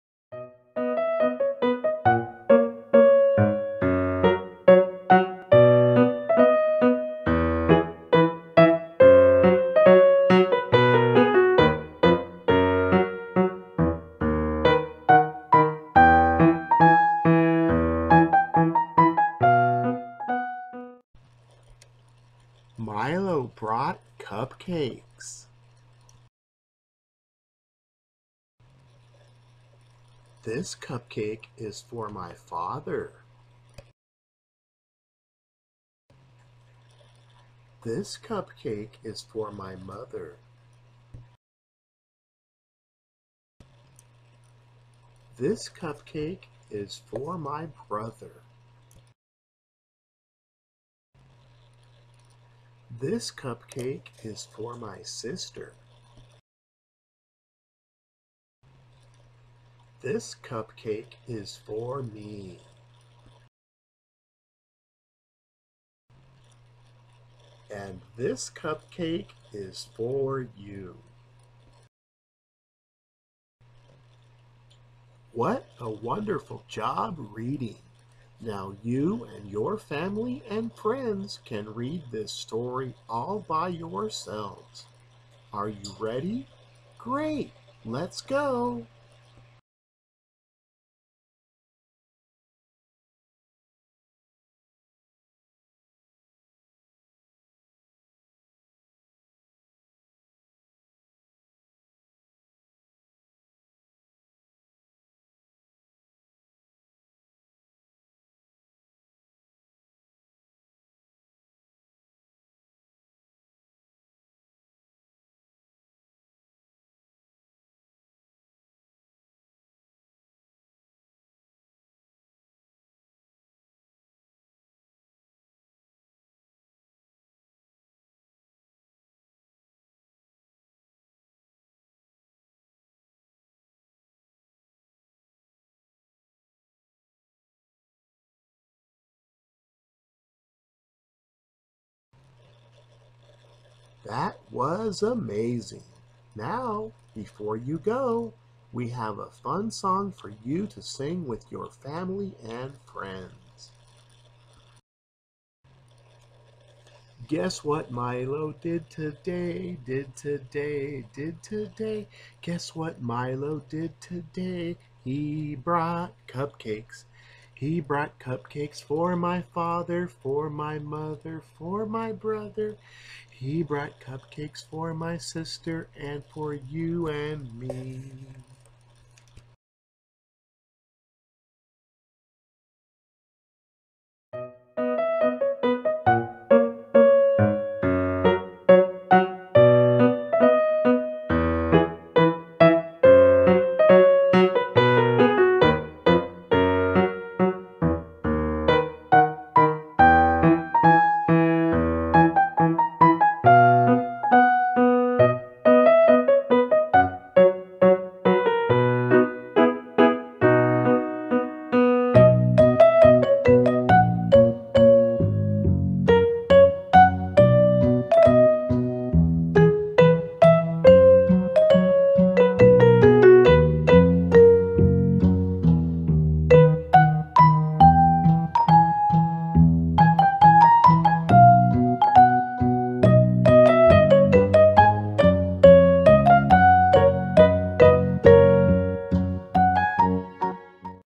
Hasemeier Early Learning Resources- Milo Brought Cupcakes!
This is an exciting and delicious story.  Your child or students can follow along with pictures as they read along.  This video is for your pre-school, pre-kindergarten, kindergarten child or students and will review basic high frequency words in a family   theme.  As your child or students read this story over and over, they will soon be able to read the repeated words by memory.  As they do, they are learning to read!  Have a great time helping your child or students grow.